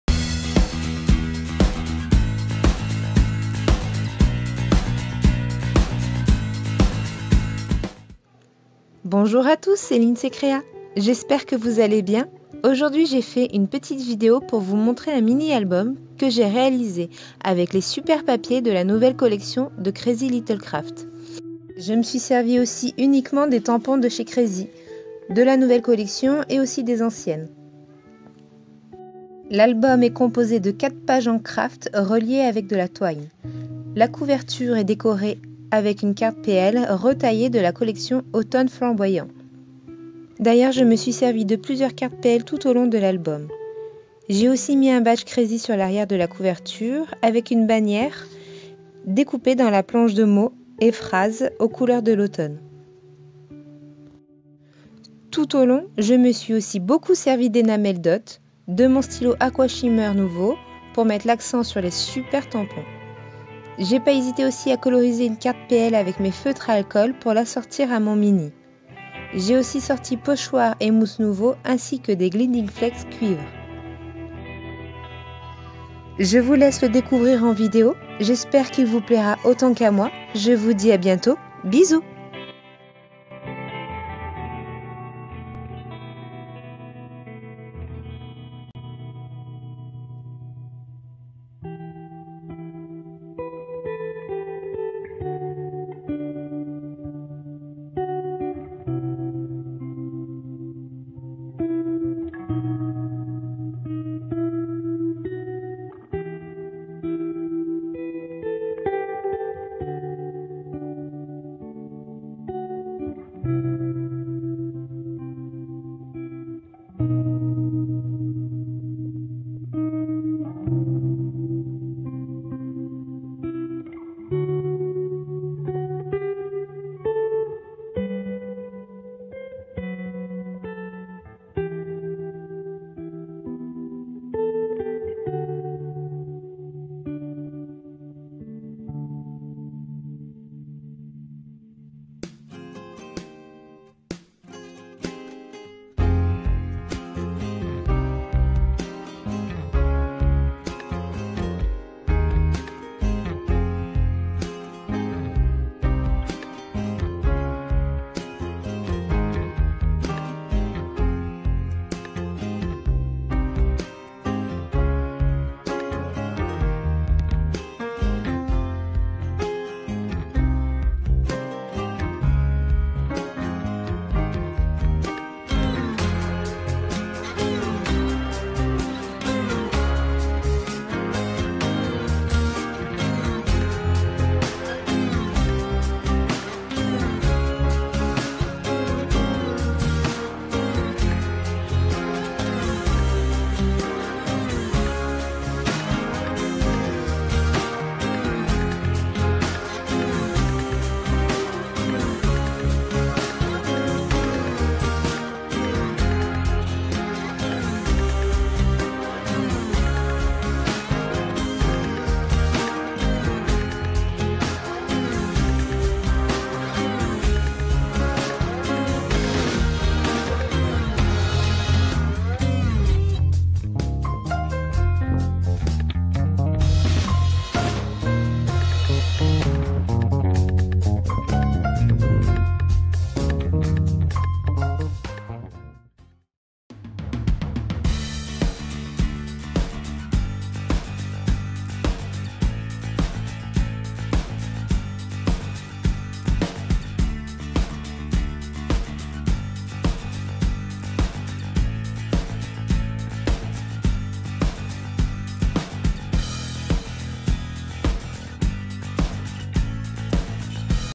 Mini album d'automne avec Lyncé Créa
Lyncé Créa nous a conçu un adorable mini album avec la collection de papier "Automne flamboyant ".
Un superbe projet plein d'astuces qu'elle vous présente dans cette vidéo !

Matériel utilisé :

Tampons

- Tampon cadeau automne 2019
- Tampon cadeau Flyer salon

Papiers

Déco & Outils